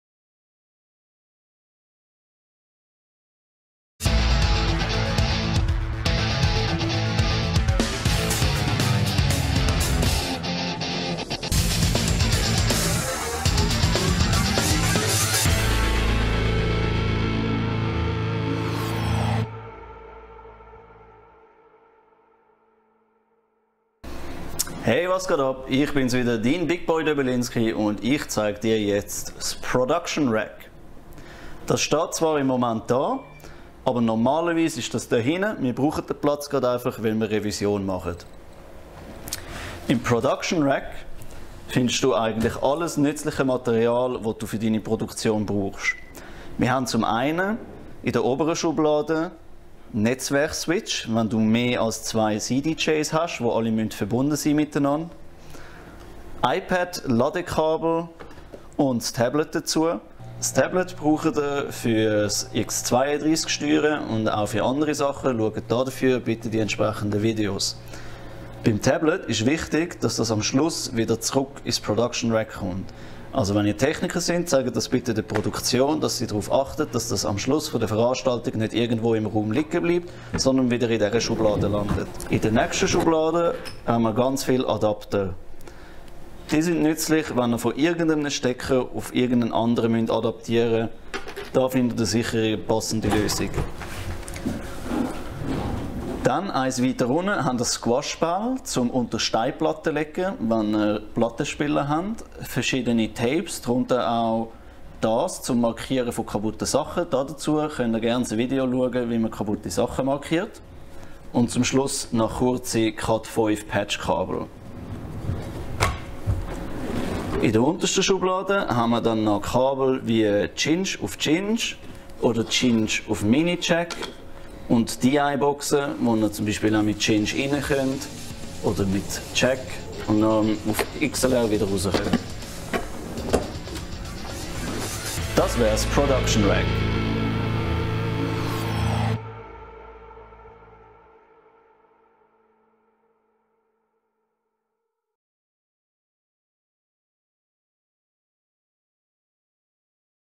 Technik IGRF_Tutorials: ProductionRack
Bist du abendverantwortliche Person oder Techniker*in in der IG Rote Fabrik und hast bei deinem Anlass technische Probleme?

In diesem VideoTutorial, extra für dich, das Thema:
Was befindet sich in unserem Production Rack und für was können diese Sachen genutzt werden.

    Netzwerk-Switches
    Tablet
    Kabel
    Adapter
    Tapes
    DI-Boxen

In der Reihe «IGRF VideoTutorials» produzieren die Technik IGRF & fabrikTV leicht verständliche Video-Tutorials mit unserem Experten Big Boy Döbelinsky.

Zu weiteren Themen wie:
etc.

Zu sehen bei fabrikTV.